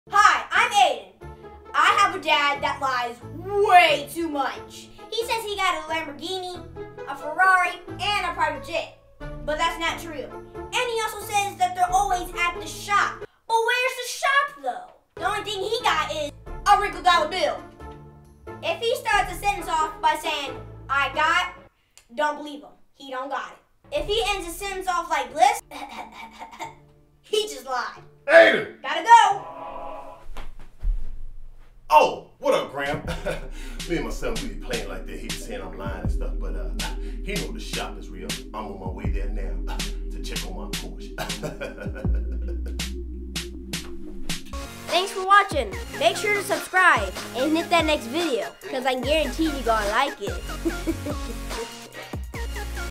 My Dad Lies (funny skits) | Wright Films
Ayden exposes his dad lies and crazy things he claims to have.